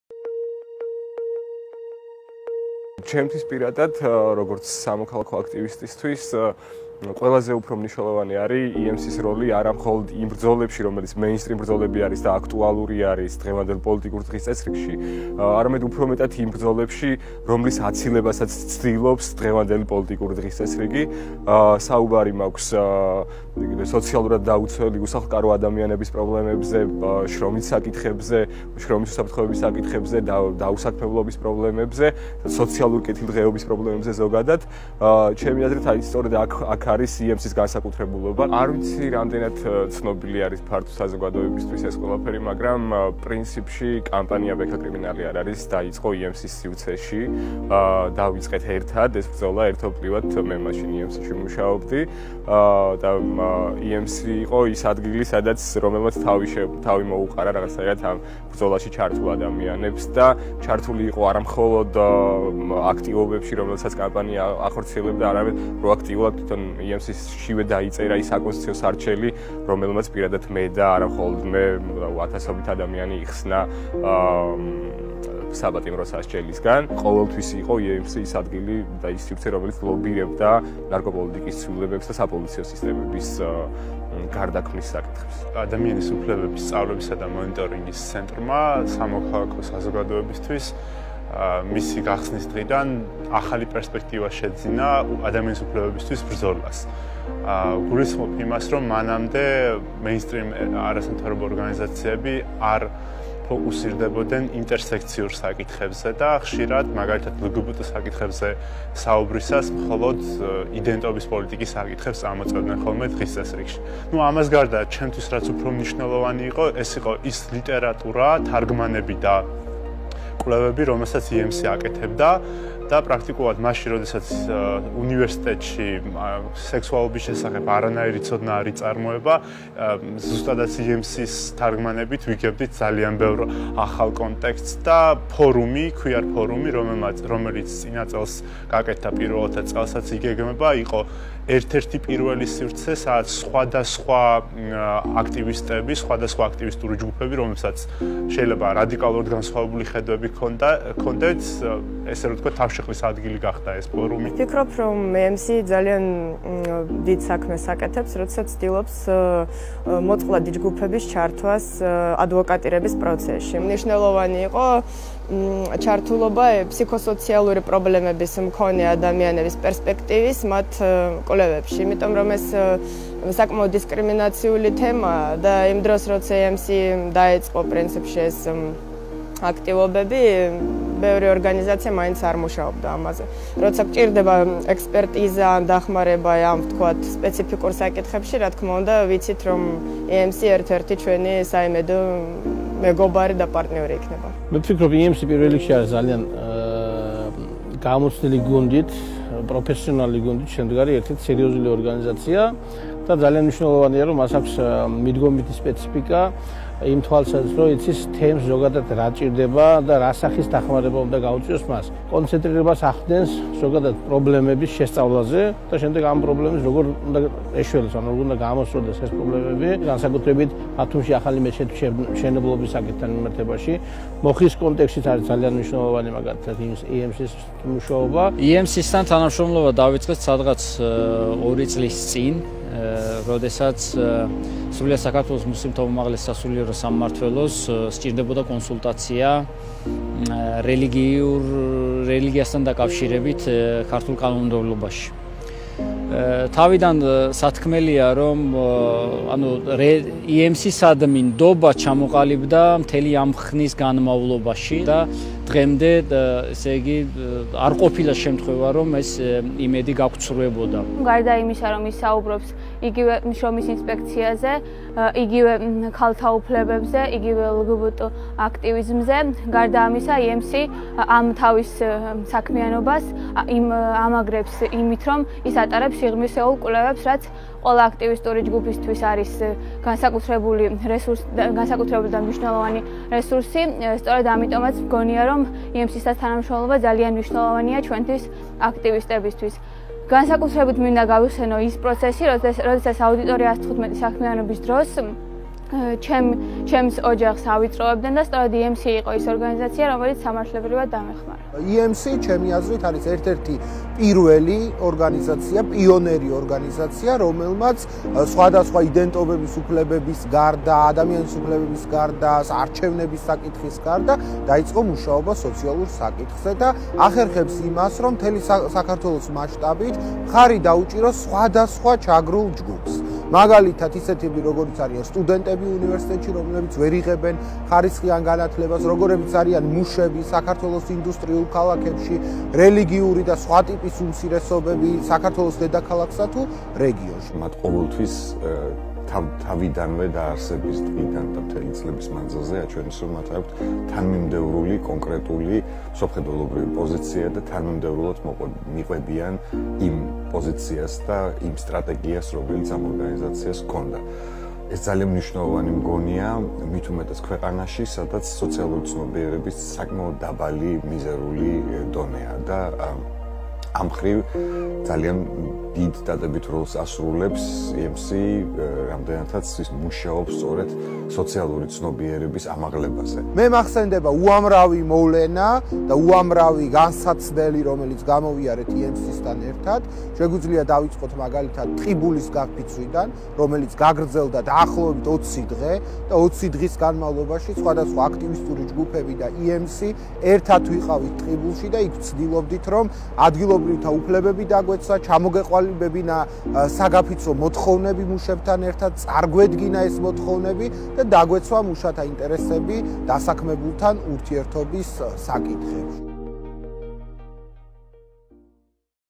რას ფიქრობენ აქტივისტები EMC-ს შესახებ
სხვადასხვა თემის წევრები, აქტივისტები და თანამოაზრეები გვიზიარებენ შეხედულებებს EMC-ს საქმიანობის შესახებ.

2012 წლის 28 ნოემბერს, EMC სხვადასხვა გამოცდილების მქონე აქტივისტების მიერ დაარსდა. დაარსების დღიდან, ორგანიზაცია ცდილობს იმუშაოს მარგინალიზებული და დისკრიმინირებული ჯგუფების უფლებების დაცვის, სამართალდამცავი ორგანოების სისტემური რეფორმების, სტრუქტურული ძალადობის და სოციალური ჩაგვრის საკითხებზე.